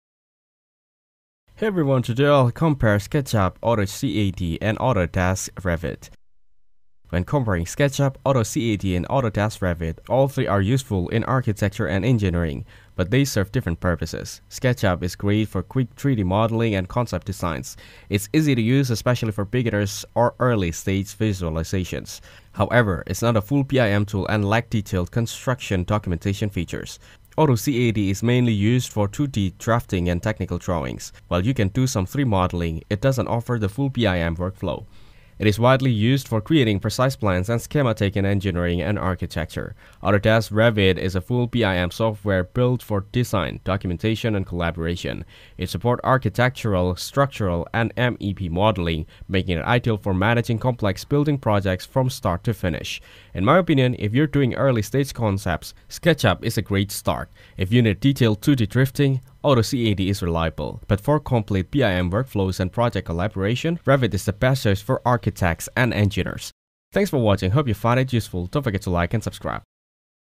SketchUp vs AutoCAD vs Autodesk Revit (2026) - Don’t Choose The Wrong One
SketchUp vs AutoCAD vs Autodesk Revit (2026) - Don’t Choose The Wrong One SketchUp vs AutoCAD vs Autodesk Revit – if you're an architect, engineer, or construction professional, choosing the right BIM software is crucial in 2026. In this video, we compare these three powerful tools to help you understand which one aligns best with your design goals, workflow, and project needs. Whether you're looking for easy 3D modeling with SketchUp, precise 2D drafting with AutoCAD, or full-scale BIM integration with Autodesk Revit, this comparison covers it all. From user interface and learning curve to collaboration features and industry usage, get a complete breakdown of SketchUp vs AutoCAD vs Autodesk Revit and make the smartest decision for your architectural or engineering journey.